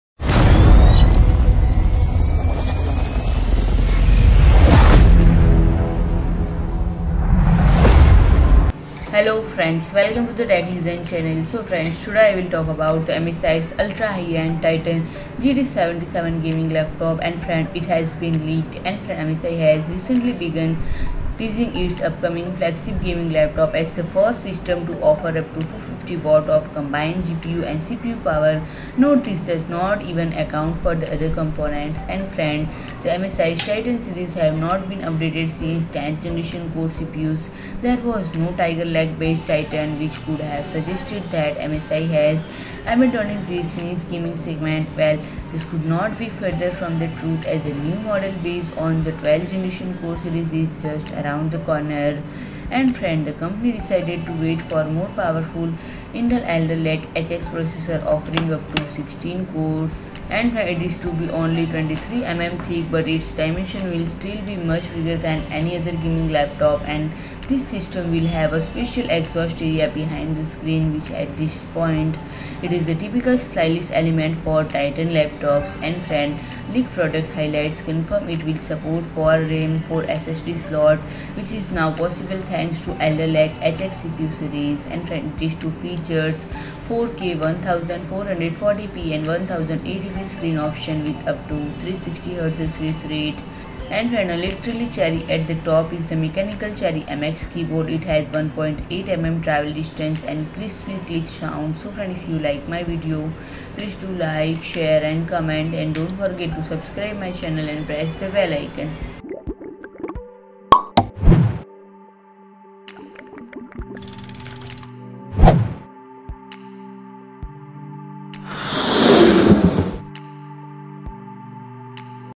MSI TITAN GT77 Ultra High-end Gaming Laptop Launched - Explained All Spec, Features & More
MSI has recently begun teasing its upcoming flagship gaming laptop as the first system to offer up to 250W of combined CPU and GPU power, note this does not even account for other components. MSI Titan series have not been updated since 10th Gen Core CPUs. There was no Tiger Lake based Titan, which could have suggested that MSI is abandoning this niche gaming segment. Well, this couldn’t be further from the truth, as a new model based on the 12th Gen Core series is just around the corner. The company decided to wait for more powerful Intel Alder Lake-HX processors, offering up to 16-cores. MSI GT77 is to be only 23 mm thick, but its dimensions will still be much bigger than any other gaming laptop. This system will have a special exhaust area behind the screen, which at this point is a typical stylish element for Titan laptops. Leaked product highlights confirm it will support four RAM and four SSD slots, which is now possible thanks to Alder Lake-HX CPU series. The TITAN GT77 is to feature 4K, 1440p and 1080p screen options with up to 360 Hz refresh rate. A literally cherry at the top is the mechanical Cherry MX keyboard. It has 1.8 mm travel distance and ‘crispy click sound’. MSI has not yet confirmed when the new TITAN is to be released, but a word of advice if you were considering buying one, it definitely won’t be cheap.